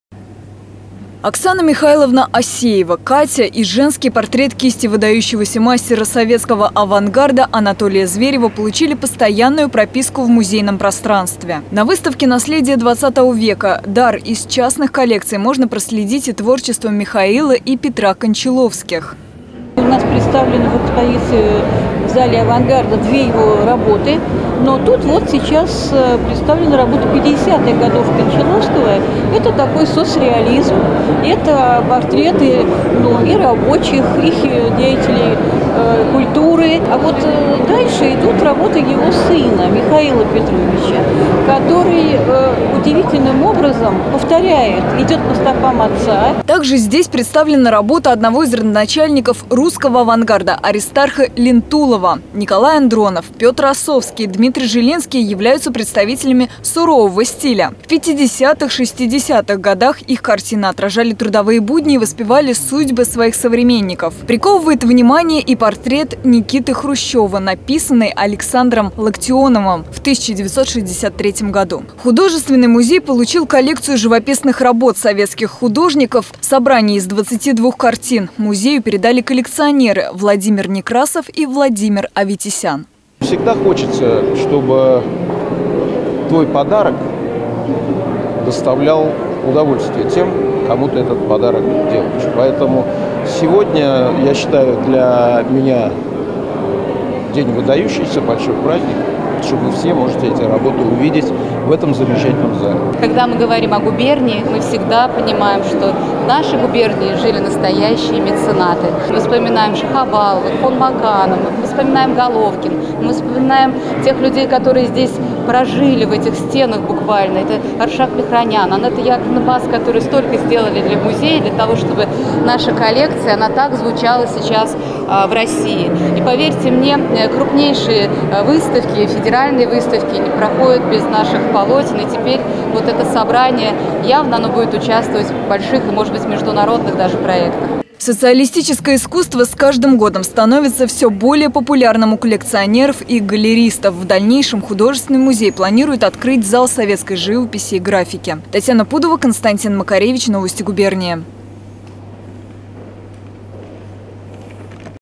Самарскому музею подарили 22 картины русских и советских художников
Меценаты передали в дар Самарскому областному художественному музею 22 картины русских и советских художников. Их можно увидеть на выставке "Наследие XX века. Дар из частных коллекций".

Среди картин - работы Петра и Михаила Кончаловского, Анатолия Зверева, Николая Андронова и других культовых советские художников-авангардистов.

Собрание из 22 картин музею передали коллекционеры Владимир Некрасов и Владимир Аветисян.

Социалистическое искусство с каждым годом становится все более популярным у коллекционеров и галеристов. В дальнейшем художественный музей планирует открыть зал советской  живописи и графики.

Подробнее о выставке - в нашем сюжете.